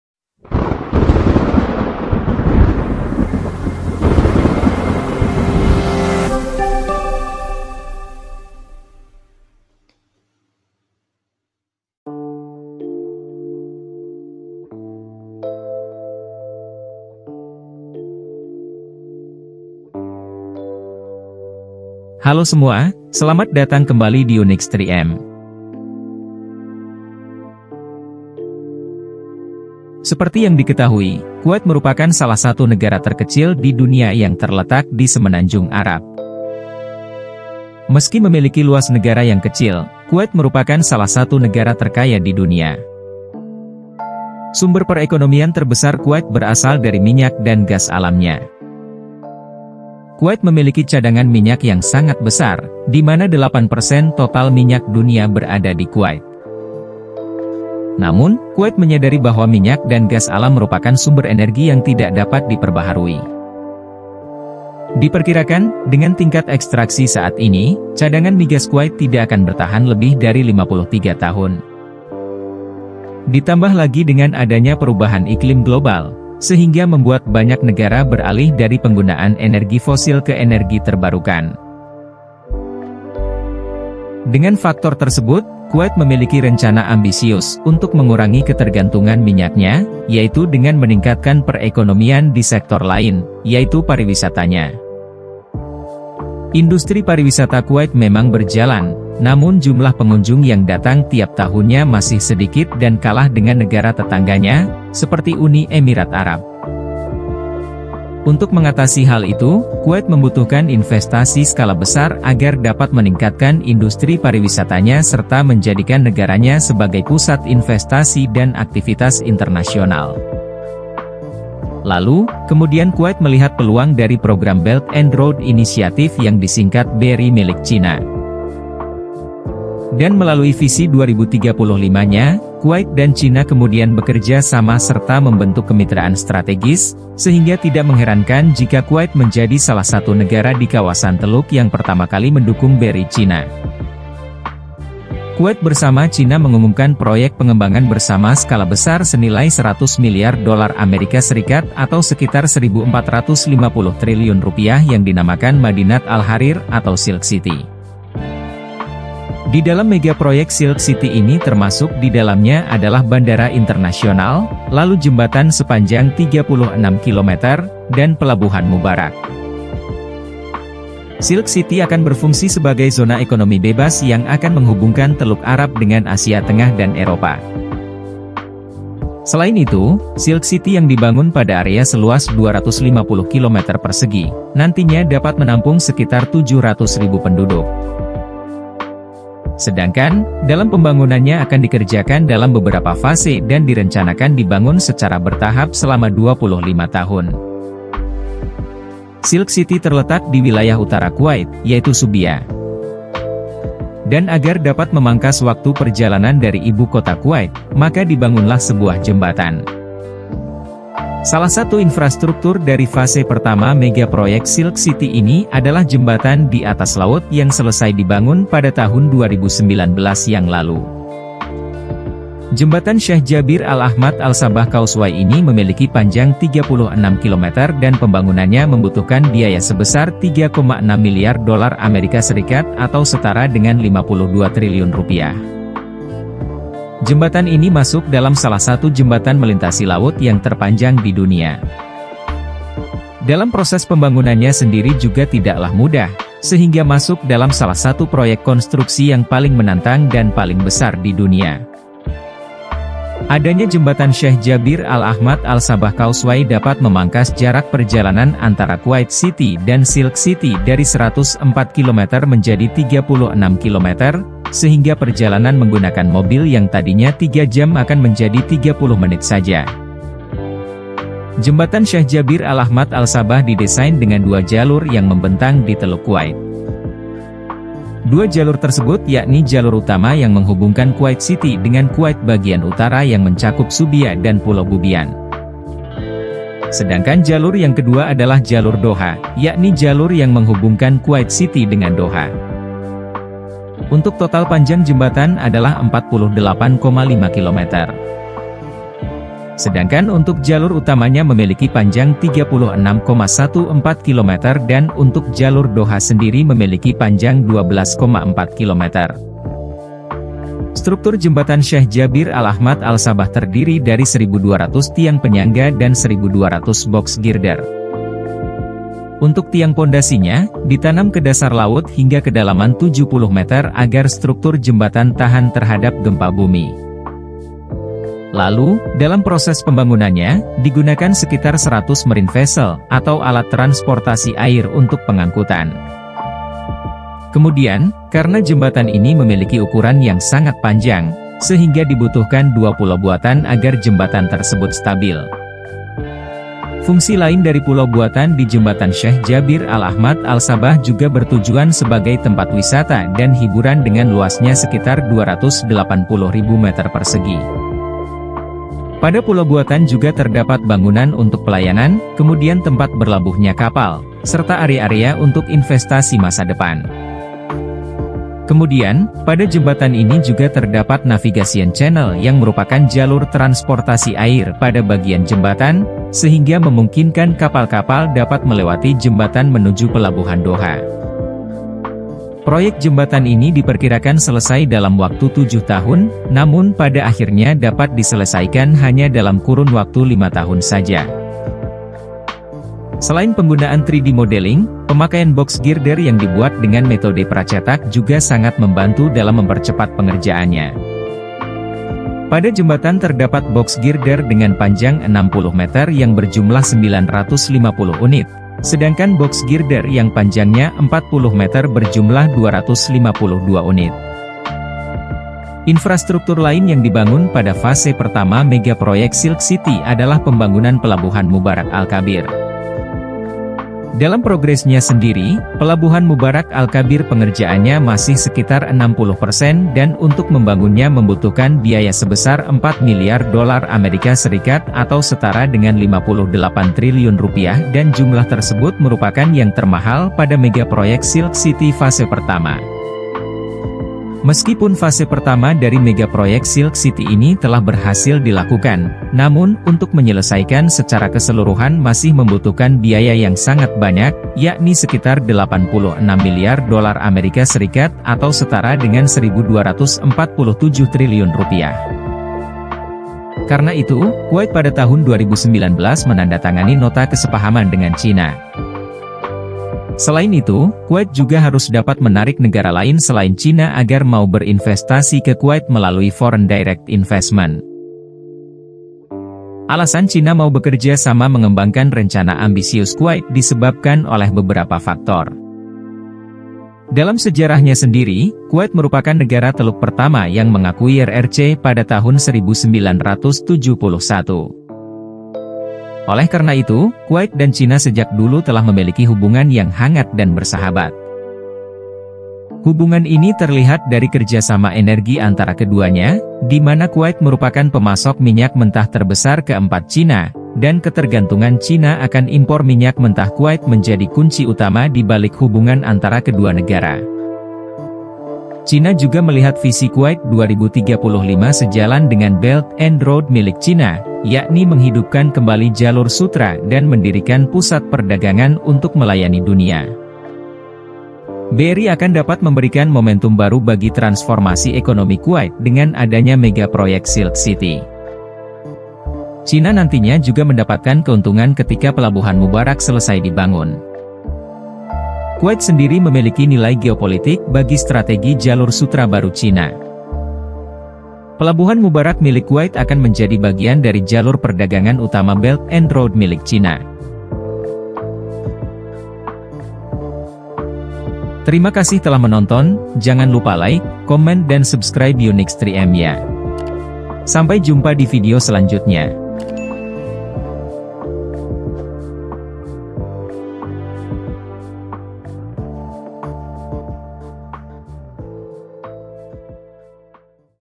Mega Proyek Silk City: Transformasi Kuwait Menuju Pusat Ekonomi dan Pariwisata
Selamat datang kembali di "UNIX 3M"! Pada video kali ini, kita akan menjelajahi mega proyek ambisius yang sedang mengubah wajah Kuwait, yaitu Silk City. Kuwait, salah satu negara terkecil di dunia, telah memulai langkah besar untuk mengurangi ketergantungannya pada sumber daya energi fosil, khususnya minyak dan gas alam. Melalui visi 2035 dan kemitraan strategis dengan Cina melalui Belt and Road Initiative (BRI), Kuwait beralih ke sektor pariwisata dengan proyek mega senilai 100 miliar dolar AS, Madinat al-Hareer atau Silk City.

Dalam video ini, kita akan menjelajahi salah satu infrastruktur utama dari proyek ini, yaitu Jembatan Syekh Jabir Al-Ahmad Al-Sabah Causeway. Dengan panjang mencengangkan 36 km, jembatan ini tidak hanya menjadi salah satu yang terpanjang di dunia, tetapi juga memiliki struktur yang memukau, dengan 1.200 tiang penyangga dan 1.200 box girder. Pembangunan jembatan ini bukan tanpa tantangan, dan kita akan melihat bagaimana teknologi 3D modeling dan metode pracetak telah mempercepat pengerjaannya.

Selain itu, video ini akan membahas dampak ekonomi dari proyek Silk City, bagaimana pelabuhan Mubarak Al-Kabir akan menjadi bagian integral dari jalur perdagangan utama Belt and Road Initiative Cina, dan bagaimana Kuwait berusaha menarik investasi asing selain dari Cina untuk mendukung rencana pembangunan ini.

Jangan lewatkan momen ketika Kuwait dan Cina bersatu untuk membentuk masa depan yang penuh prestasi ini. Saksikan video lengkapnya untuk mengetahui lebih banyak!